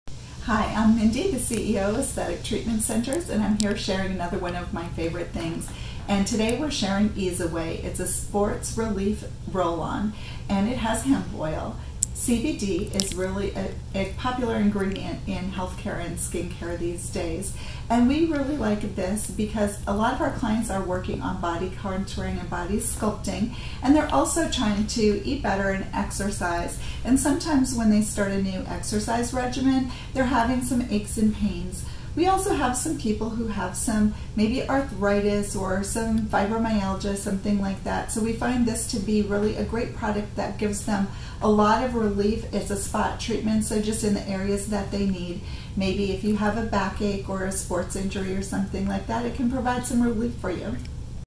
Aesthetic Treatment Centers (ATC) Mindy's favorite things Eez Away
Soothe sore muscles and pain with Eez Away. The combined power of analgesics with the added benefits of CBD make this our go to for the aches and pains of everyday life.